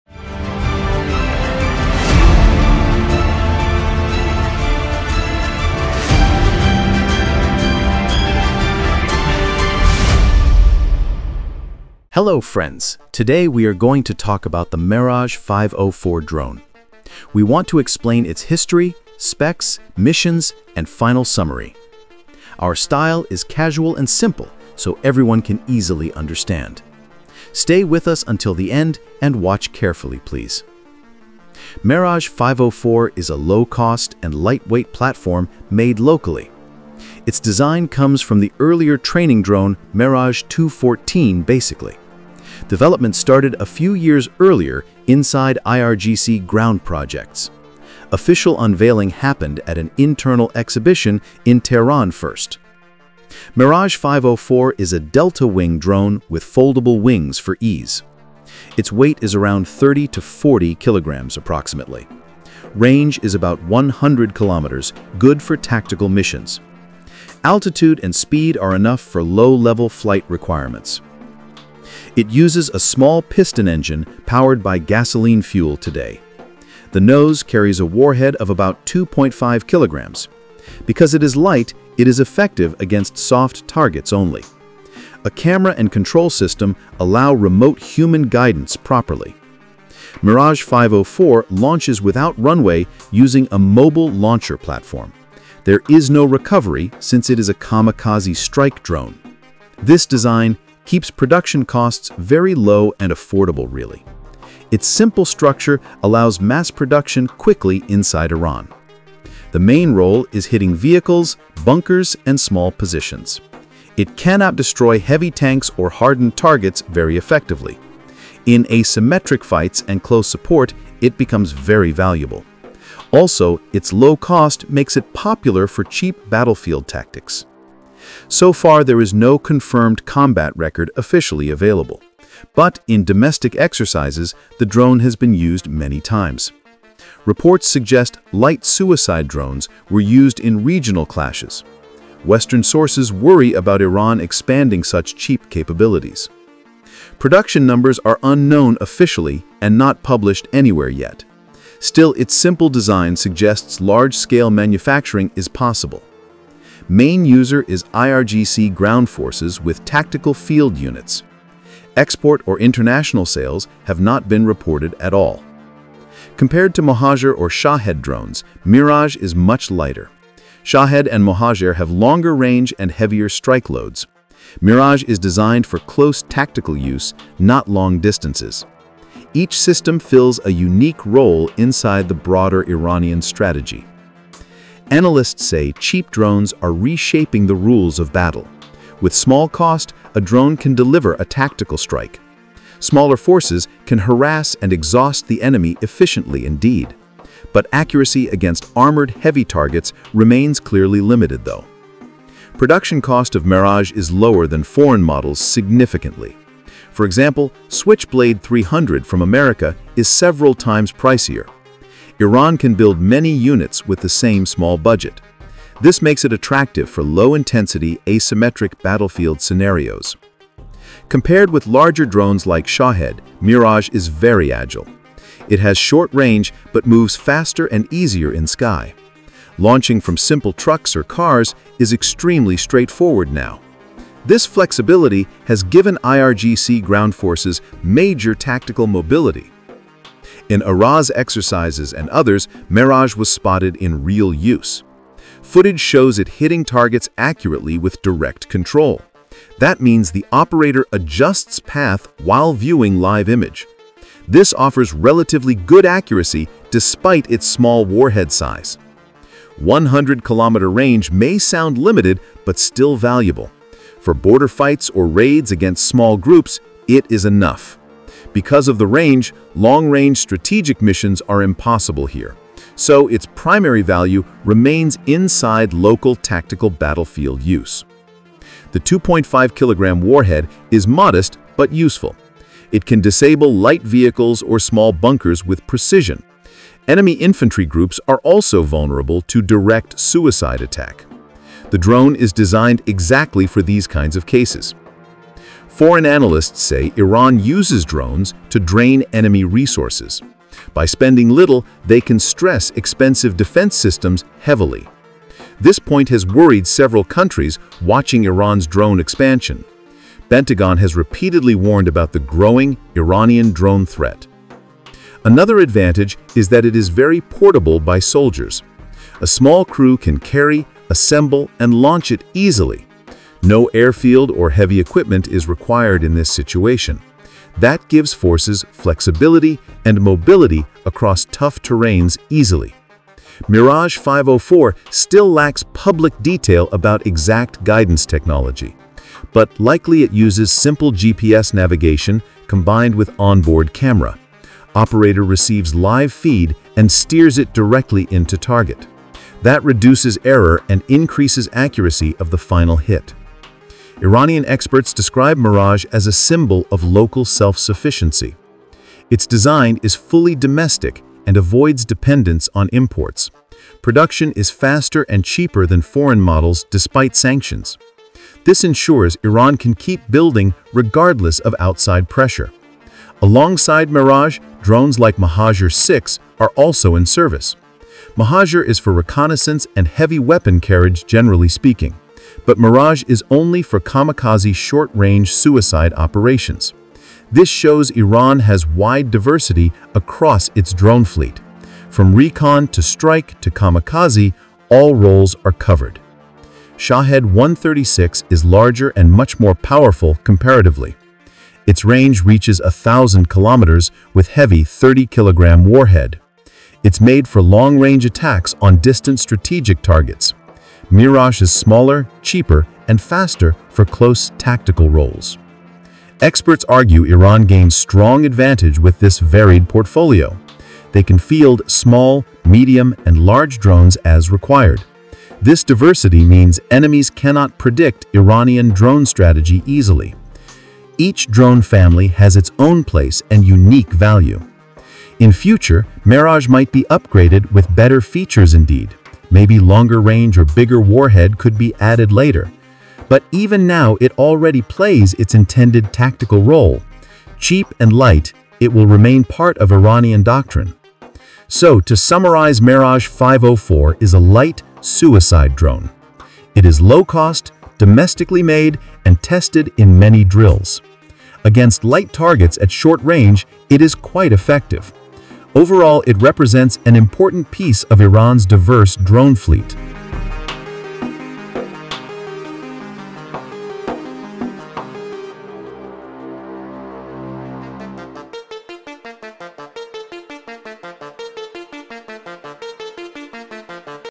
IRAN Meraj-504: The Unknown Power of Iran’s Drone Arsenal || Explained.
The Meraj-504 is one of Iran’s lesser-known drones, designed as a lightweight, low-cost suicide drone for tactical use. Its origins are based on the earlier Meraj-214 training UAV. Developed by IRGC ground forces, Meraj-504 was first unveiled quietly in internal exhibitions. With a delta-wing layout and foldable wings, it is designed for short-range operations and fast deployment.

Weighing around 30 to 40 kilograms, it uses a small gasoline piston engine and carries a 2.5 kg nose warhead. Meraj-504 does not require a runway; it is launched from a mobile launcher and has no recovery system, reinforcing its kamikaze role. Its 100 km range and simple GPS-camera guidance system make it effective for hitting light vehicles and small tactical targets.

While there are no confirmed reports of combat use, Meraj-504 has appeared multiple times in domestic military exercises like Araz. Its design allows for mass production, and its affordability makes it attractive for asymmetric warfare. Unlike larger Iranian drones like Shahed-136 or Mohajer-6, Meraj is faster, lighter, and built for close-range strike roles.

Foreign analysts have expressed concern that Iran’s ability to produce such cheap and tactical drones could disrupt conventional warfare economics. With drones like Meraj-504, Iran can target expensive enemy assets at very low cost. While not suitable for large or armored targets, it gives tactical flexibility to IRGC ground units and represents Iran’s growing drone diversification.
This video is visual created using A.I 3D Tools. It is for informational and educational purposes only.